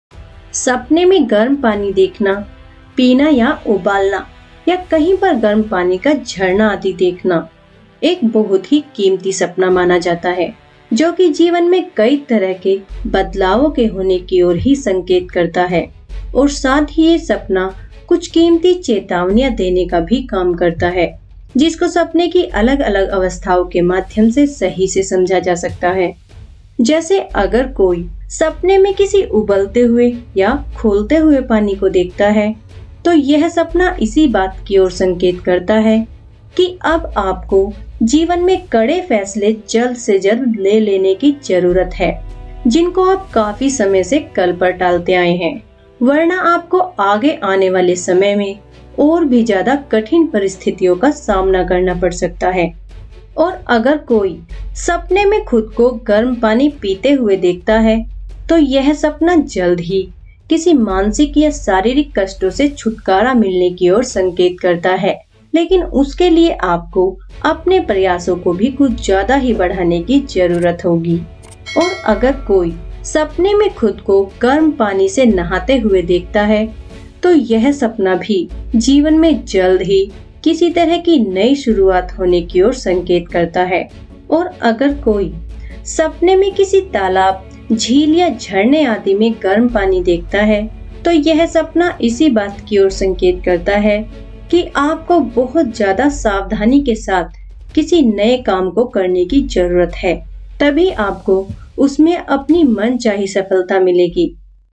Sapne me garam pani pina ya dekhna | Drinking Hot water in dream | garam pani ka sapna dekhna
Sapne mein garam pani pina ya sapna me hot water pina nahana ya dekhna kitna shubh ya ashubh hota hai.